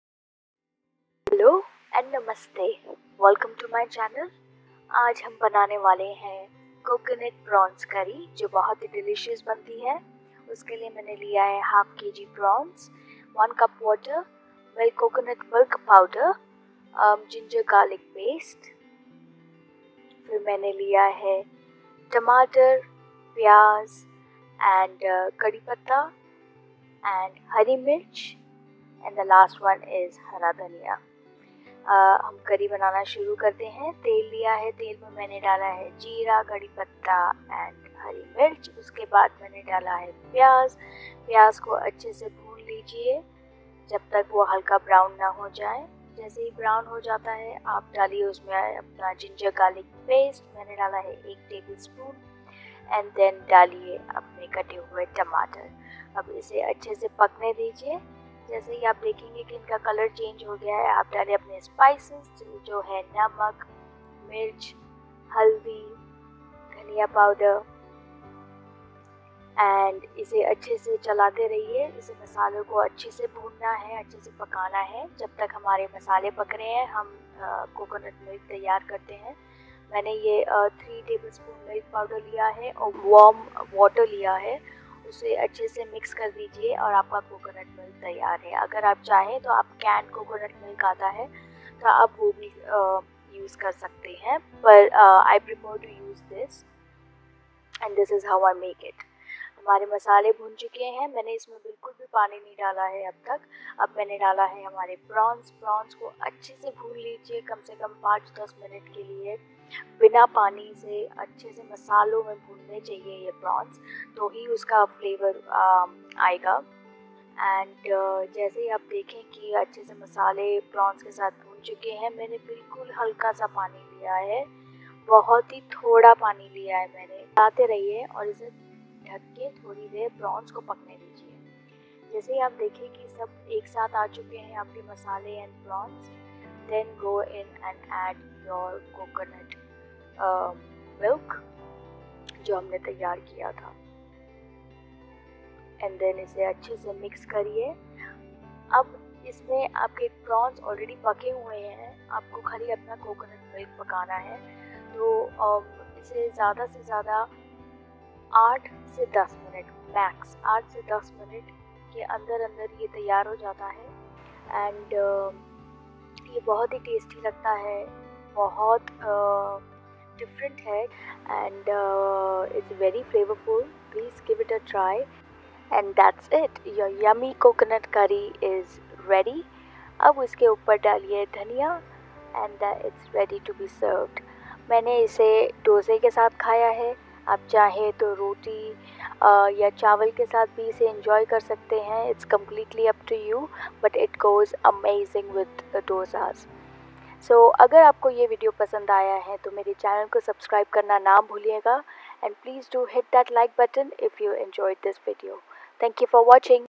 Coconut prawns curry recipe | how to make prawns curry with coconut milk | नारियल झींगे की रेसिपी
Coconut prawns curry
Hello everyone,
In today's video I will be showing you how to make coconut pranws curry in hindi.

This is a perfect Malabar style coconut prawn curry.

Restaurant style coconut prawns curry that you can easily make it at home.

Its easy to make this mouth watering restaurant style coconut prawns curry .

It take on 25minutes to prepare this delicious curry.

If you are a seafood lover you should definitely try this recipe out.

So what are you waiting for. Go get into your kitchen and try this recipe out now!

You can pair it up Dosa, Roti or Rice.

I would love to hear it from you.

Love & Light.

# नारियल झींगे की रेसिपी

झींगे करी

मालाबार करी

झींगे कैसे बनाते हैं

झींगे की रेसिपी हिंदी में